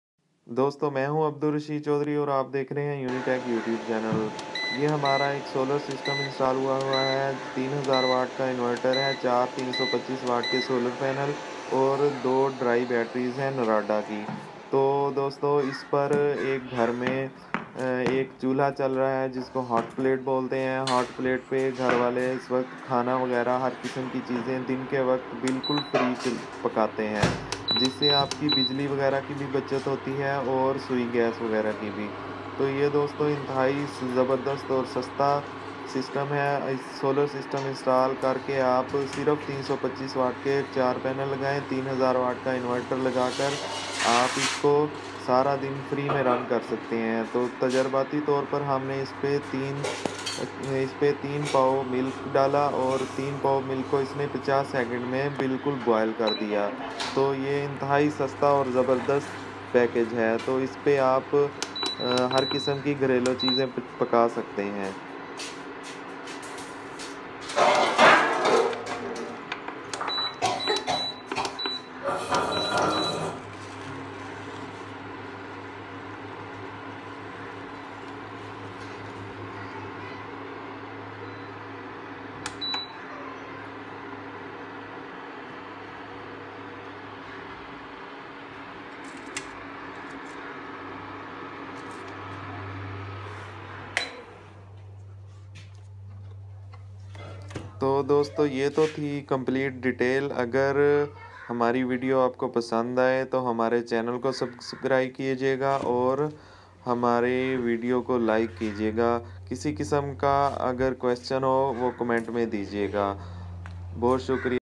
solar induction cooker 03 kw solar food cooking at home | سولر سسٹم پر کھانے پکانا ھوا آسان
Easy way to cook food on solar system | سولر سسٹم پر کھانے پکانا ھوا آسان

Solar stove Free run on solar system just install 1200 watt solar panels 2.2kw solar inverter and 2 batteries Cook everything free of cost one time investment